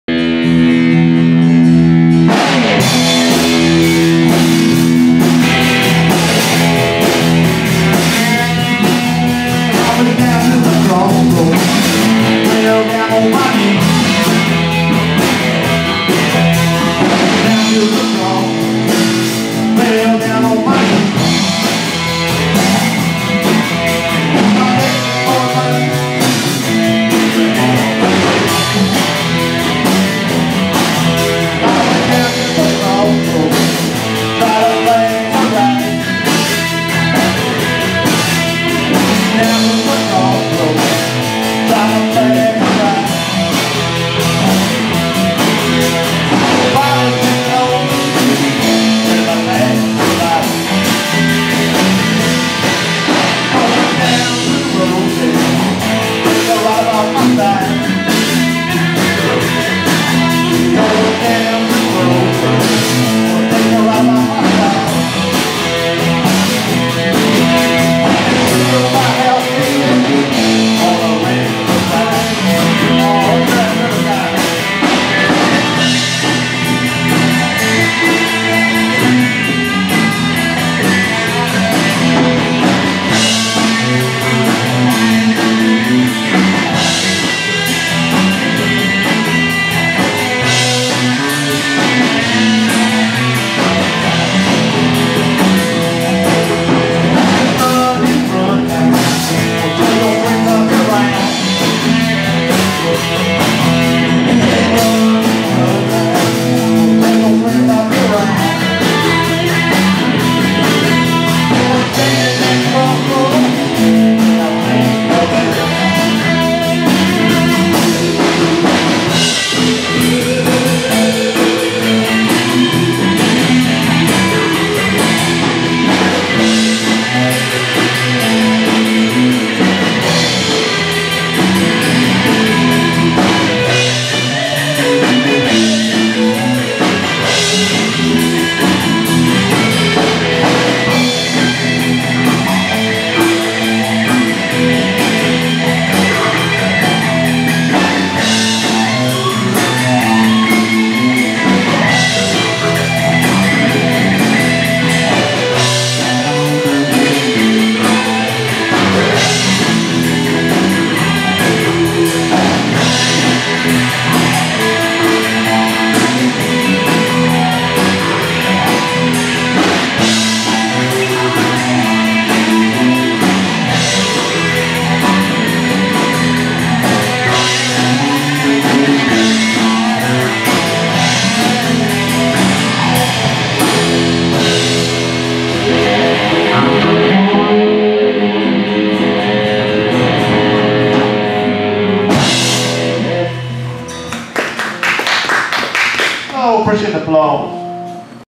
Put the pedal to the metal and head to the CrossRoads Family Tradition Band of Kentucky
The Family Tradition Band of country providing mayhem on Crossroads at the AmVets in Henderson Ky on September 8, 2018.